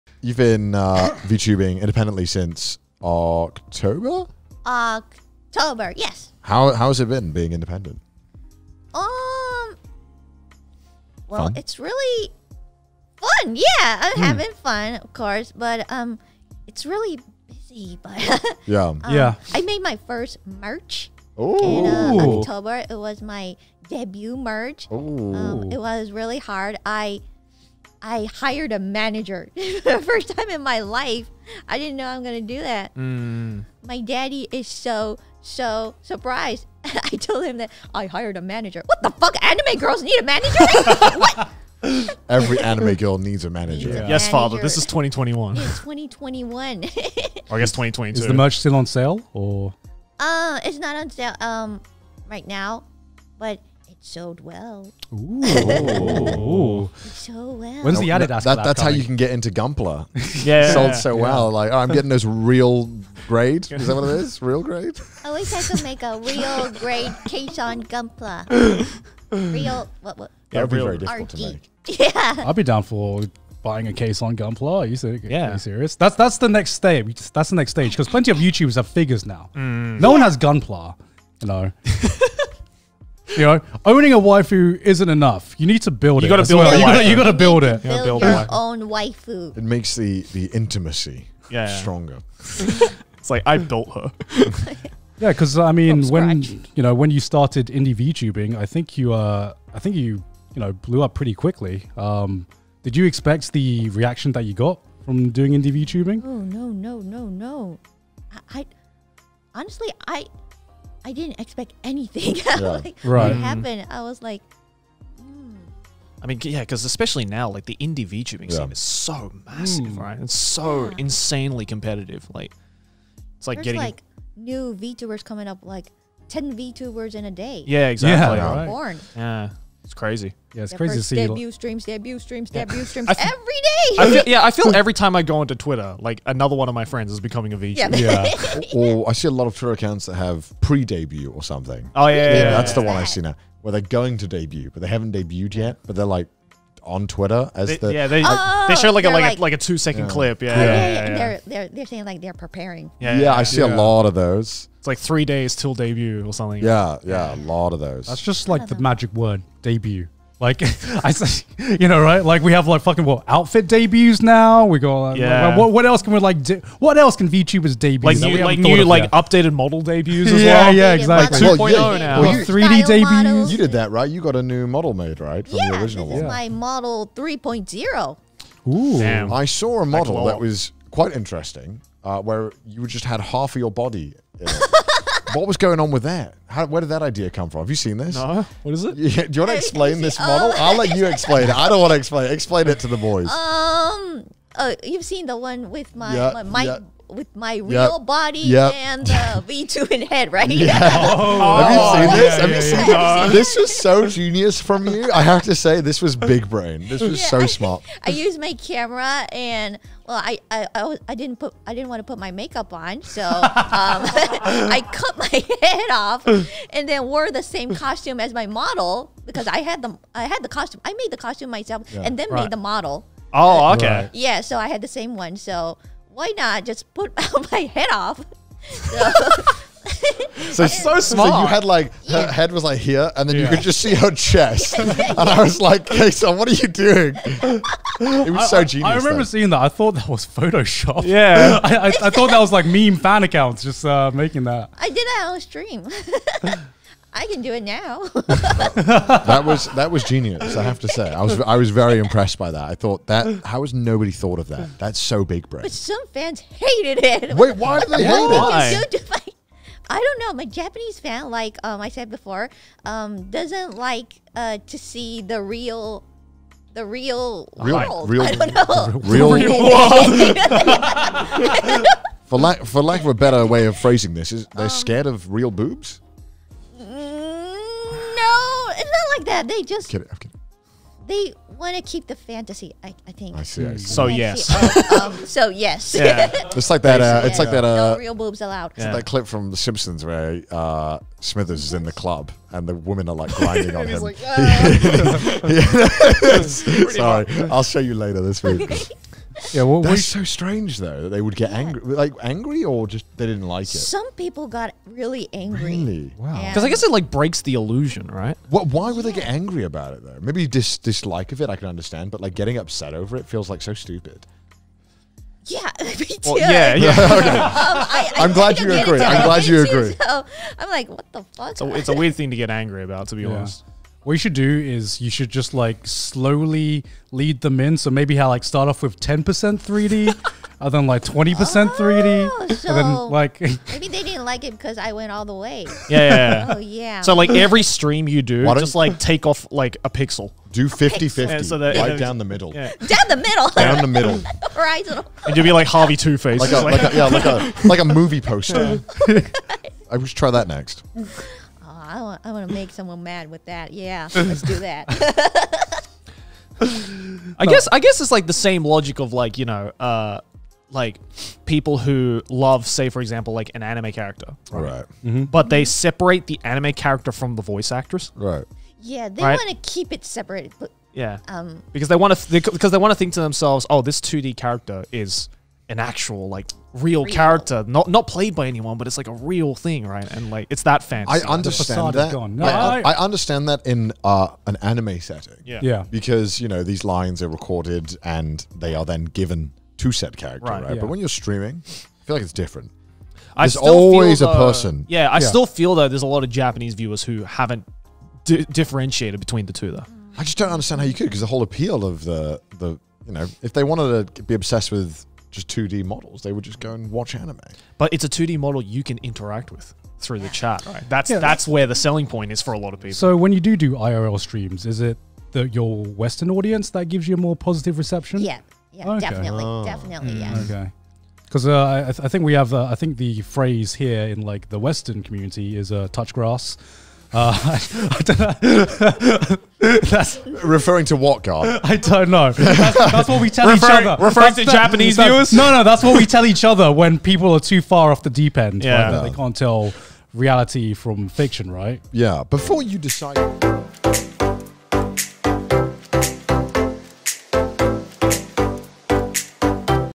【Kson】關於V皮與中之人的關係 總長是這樣想的【全熟：第十一部分】
本頻道每週2、4、6更新(ㆁωㆁ)
未經授權禁止轉載至任何其他網站(ノಠ益ಠ)ノ

全熟進度11/? 我...我詞窮了

哈威丹特：DC漫畫裡的雙面人 可以看看 《蝙蝠俠 黑暗騎士 》